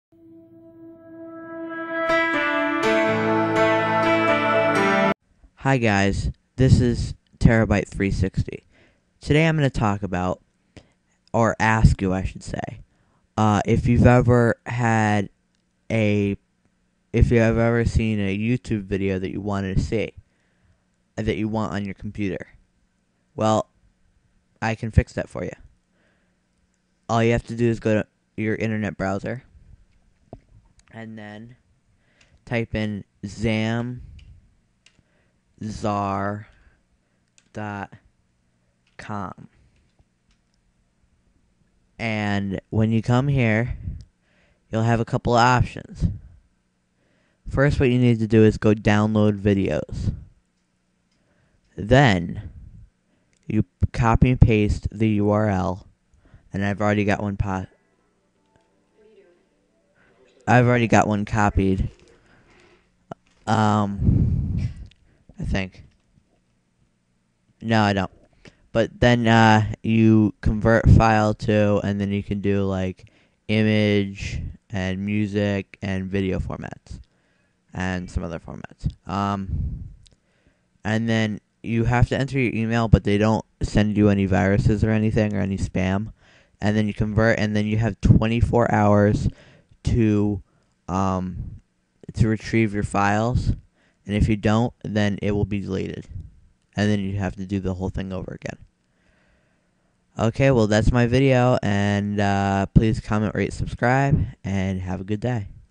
How to use Zamzar and Download Videos for Free!
this video shows you how to use Zamzar and using Zamzar to download videos